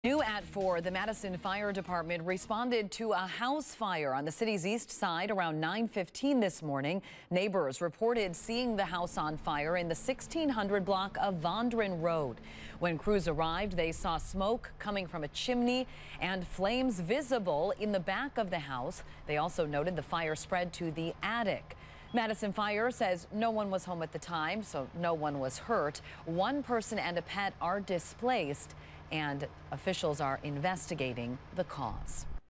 One displaced after Madison east side house fire
The fire, on the 1600 block of Vondron Rd., started shortly after 9 a.m. Wednesday, the Madison Fire Department explained.